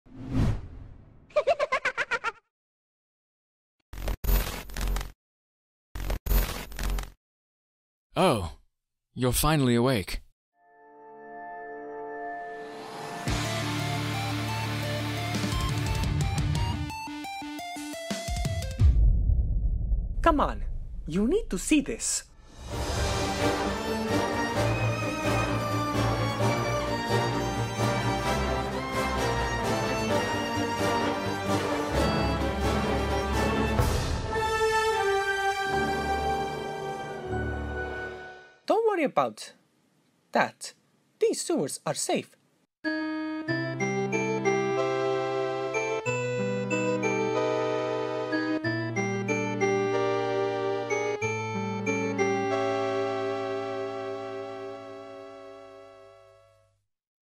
What's Coming to Chaotic Smiles Productions...
It's E13 Time! Unfortunately, I don't have anything neat to share aside from teasers. Yes, Ep. 9 is currently being worked on but with work in the way, it may take a while. So, I hope to aim for a Late Fall release. As for Ep. 10, I'm certain it'll be out by next week. And yes, Ep. 10 will be the last episode for ACT 1. We're finally coming to a close to an arc and branching upon something new...

For funnies, we got the next blooper reel! As you may have noticed, it looks... different. Given that I'll be busy, I've decided to not only shorten it but also give it a new style that I can work it. That way you can still have your blooper reels and I have time for my other projects! Running two channels can be difficult @u@

Speaking of channels, did I ever mentioned that I'm doing an Abridged series? Tell ya friends

With that being said, see you all soon~!